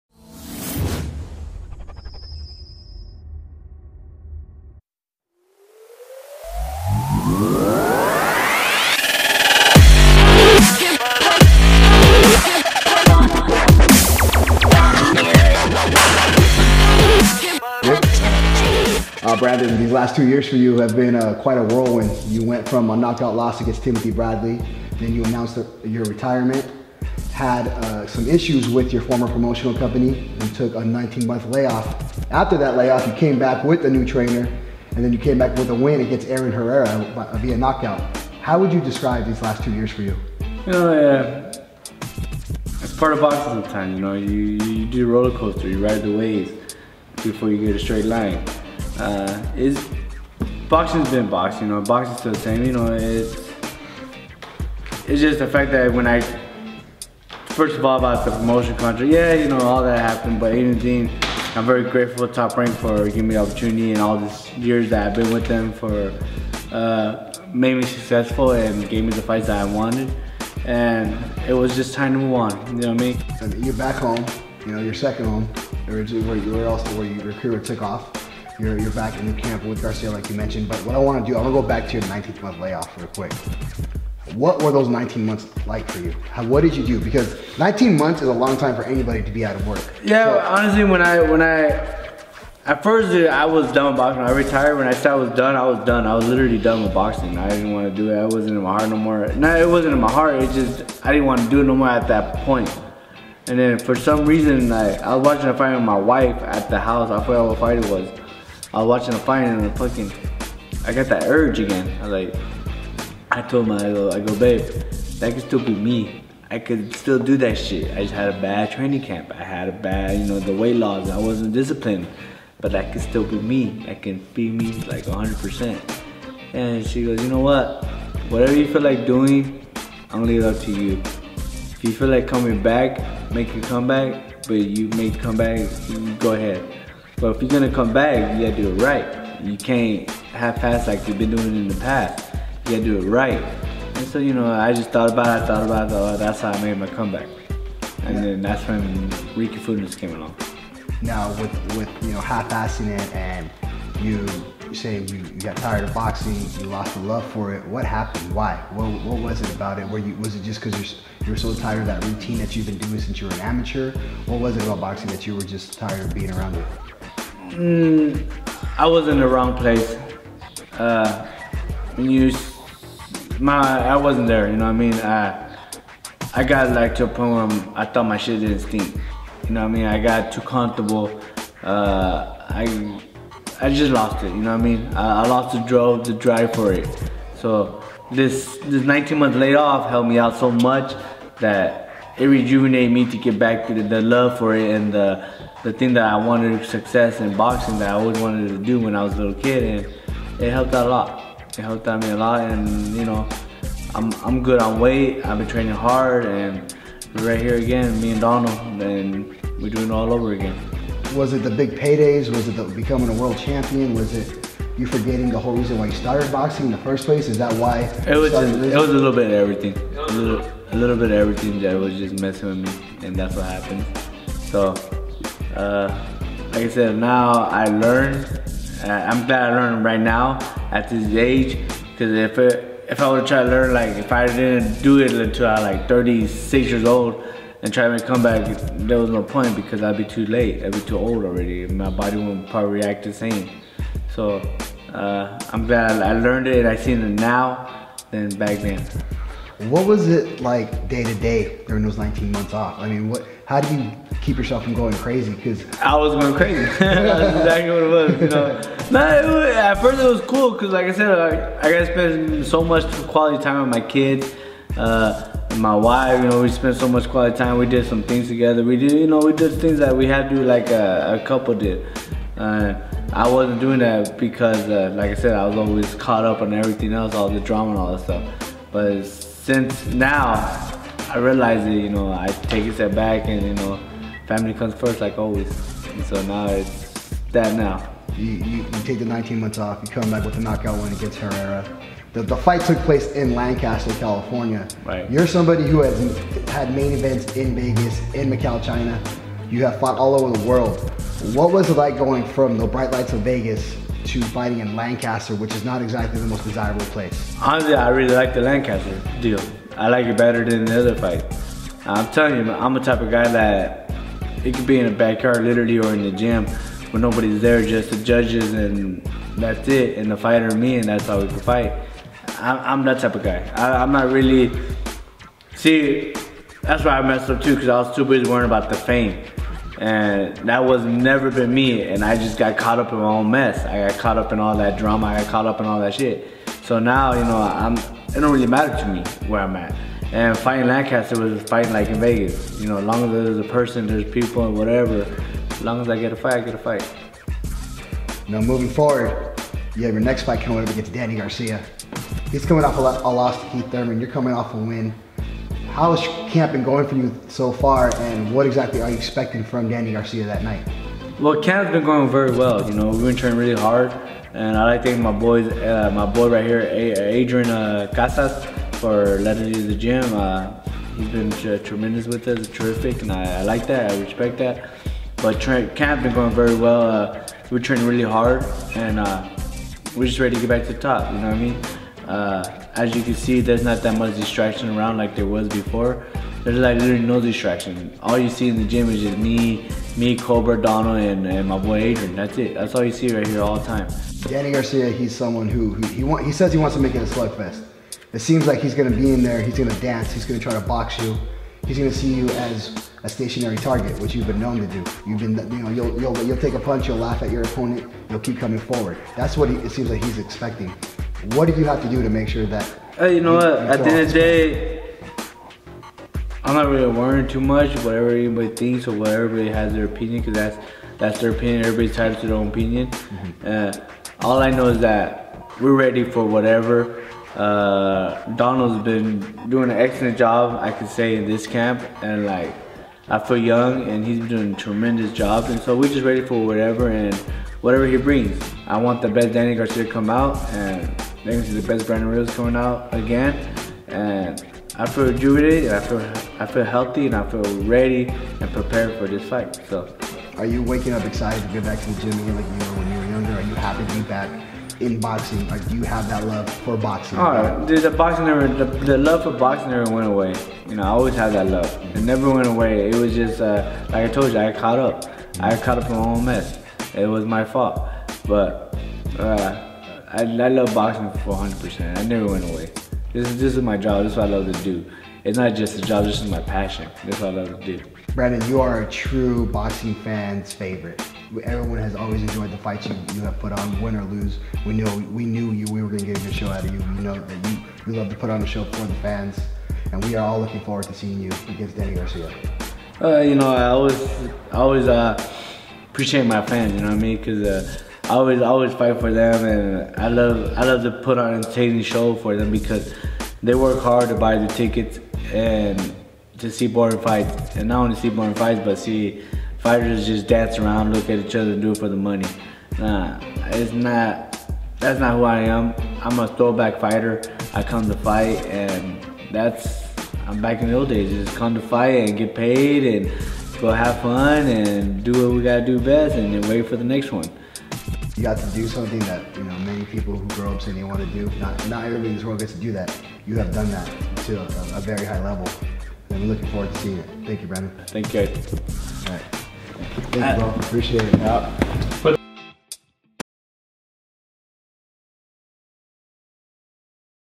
"Bam Bam" Brandon Rios on his big return vs Danny Garcia
"Bam Bam" Brandon Rios opens up camp to Supreme Boxing for his fight with Danny "Swift" Garcia, Saturday, February 17, 2017 from the Mandalay Bay Casino in Las Vegas, Nevada. Live on Showtime
Interview by Daniel Quiñones @danielq_reporting
Video & Edit by Luis Mejia @lmejiaphotography